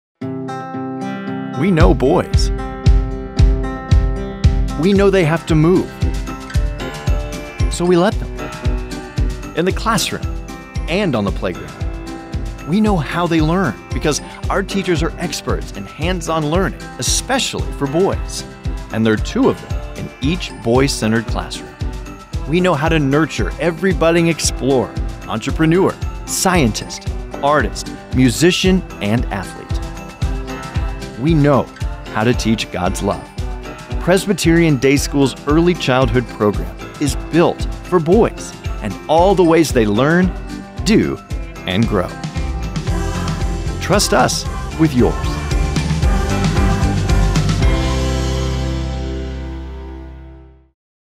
We Know Boys - Presbyterian Day School
At Presbyterian Day School, we know a better way to reach a budding explorer, scientist, or chef. We let them move, pepper us with questions, and think out loud.

This isn’t just a boys’ school. It’s a school built for boys and all the ways they learn, grow, and do.